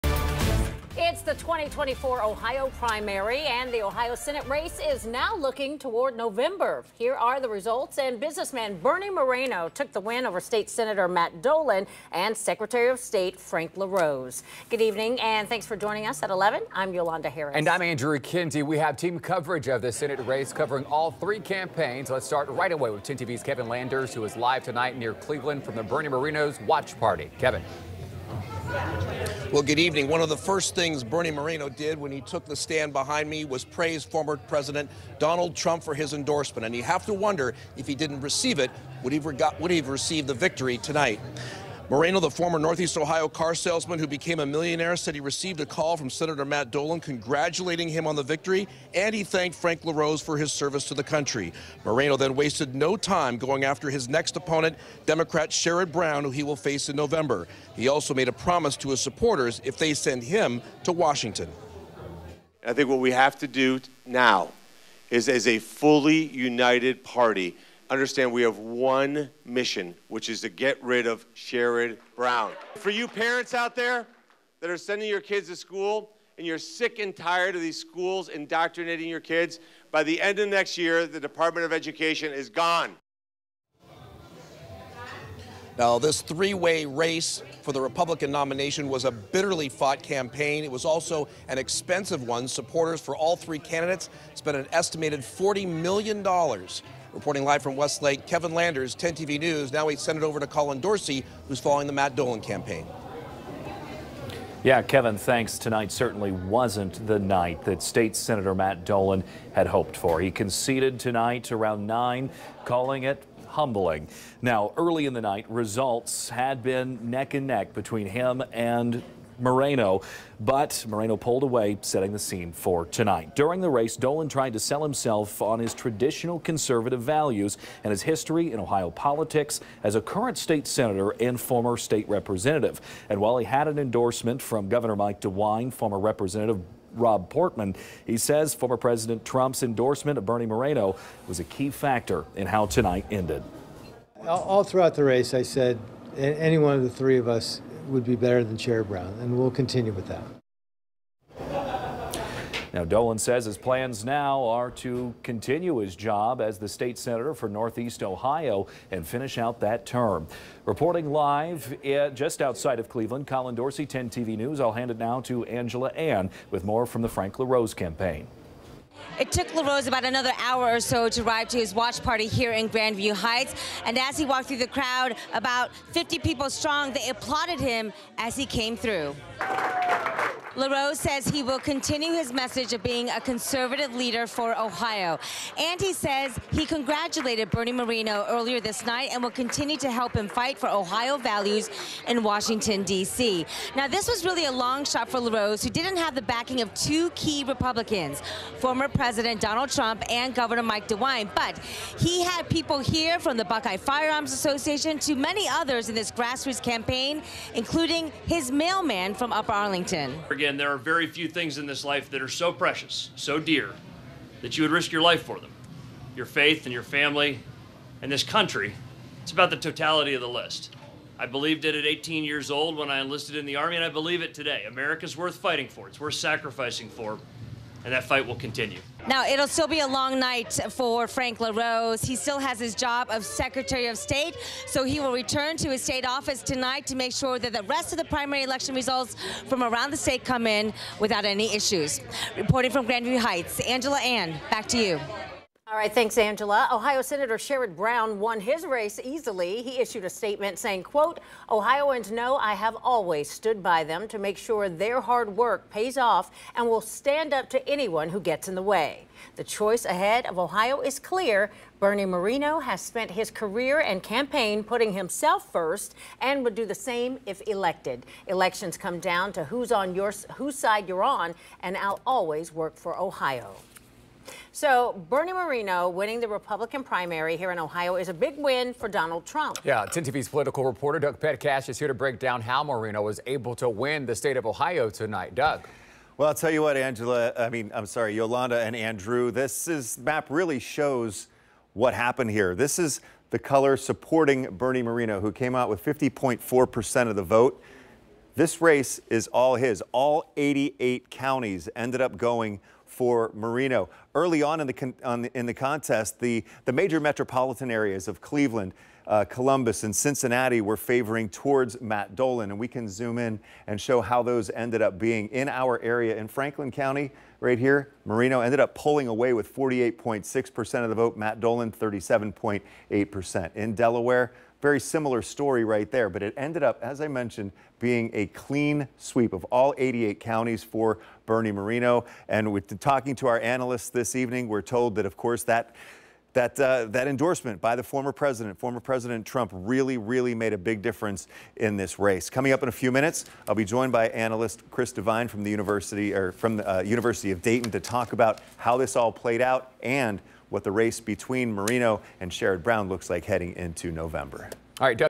Ohio Primary Election: Bernie Moreno projected to win Republican US Senate race
Moreno will take on incumbent Democratic Sen. Sherrod Brown in the general election on Nov. 5.